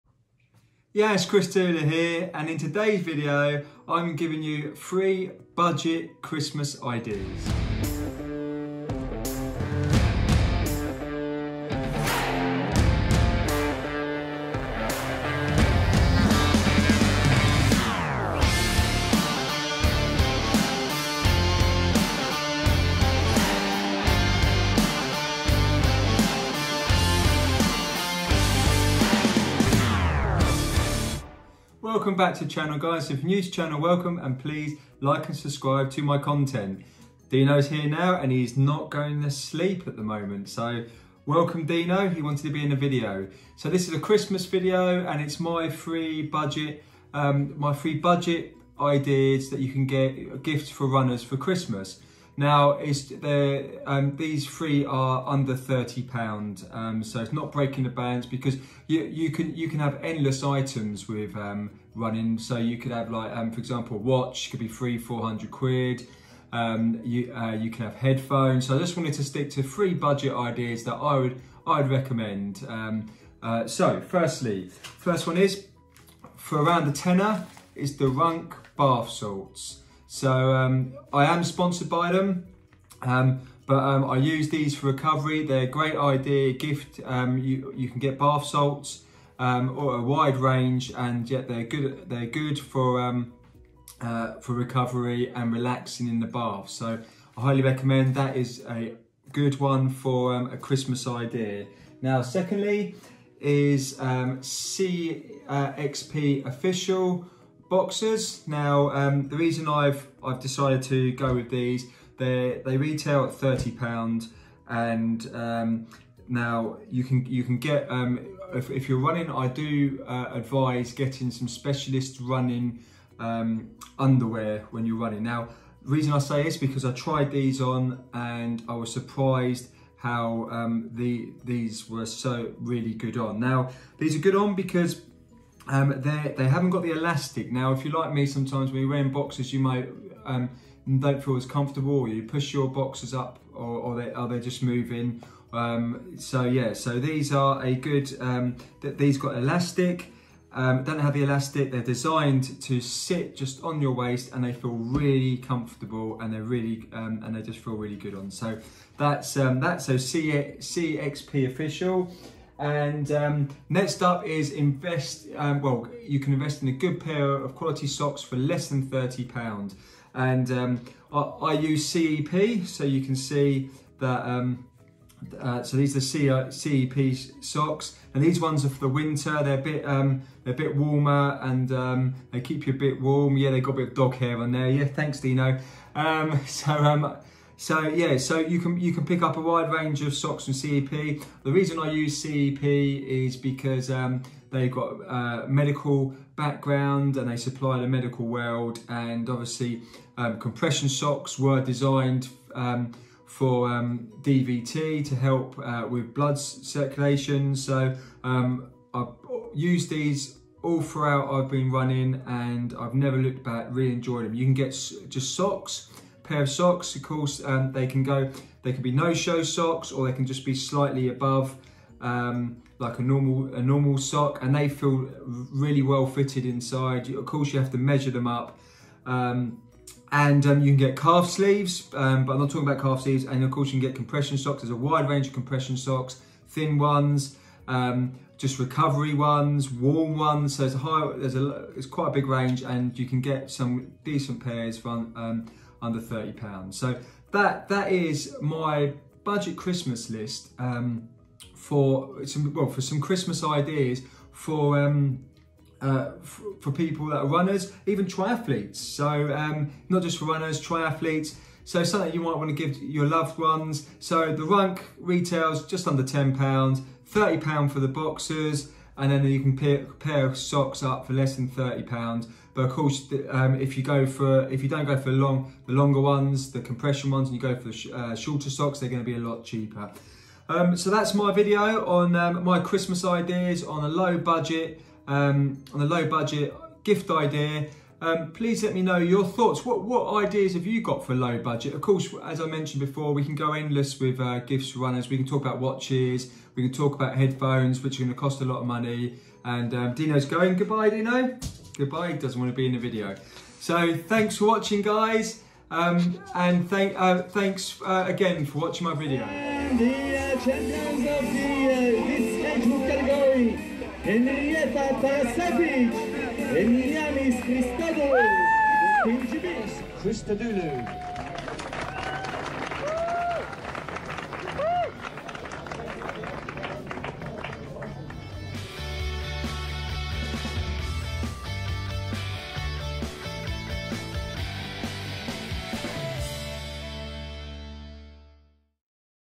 My 3 budget Christmas gift ideas for runners
I come up with three budget Christmas ideas for runners. What budget gift ideas do you recomend?